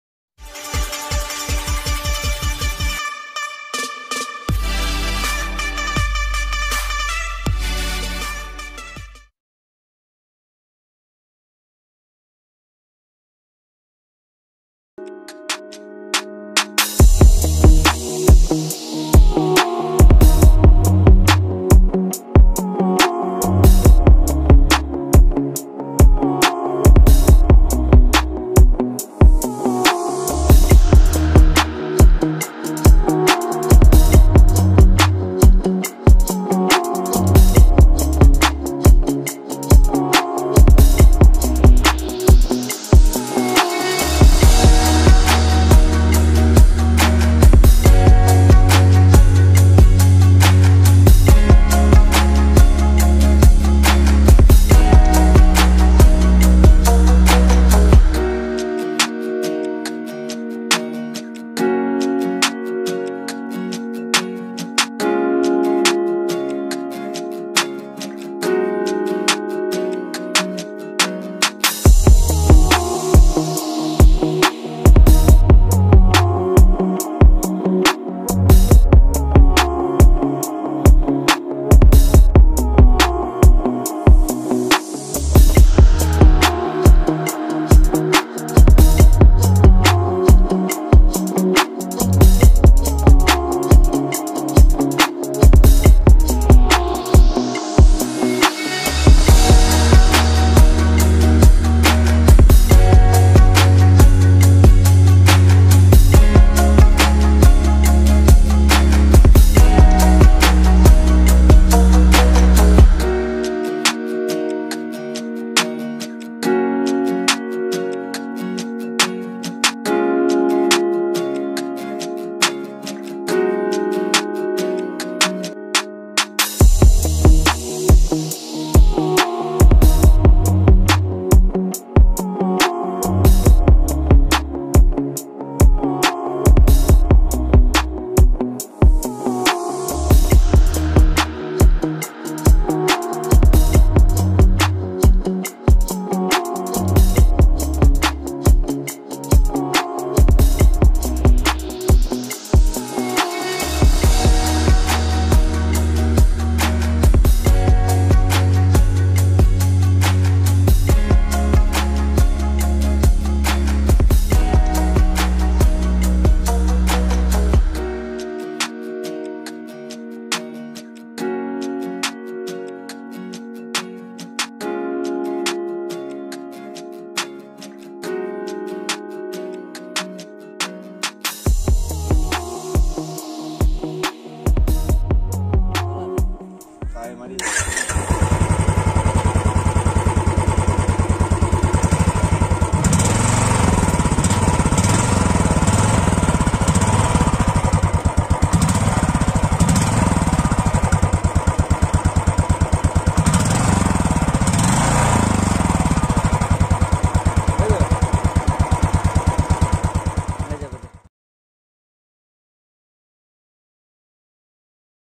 ROYAL ENFIELD CLASSIC 350 CHROME finish cinematic shot || bullet 350
hi viewers this is pranab sarkar.

i m very new to YouTube journey .

so support me how much you can .

love from assam (🇮🇳India) .

in this video im showing you a normal

cinematic shot of ROYAL ENFIELD CLASSIC 350

CHROME COLOR..

hope you like the video.. thankyou..

1- editing:- kinemaster

2- camera:-redmi poco m2 pro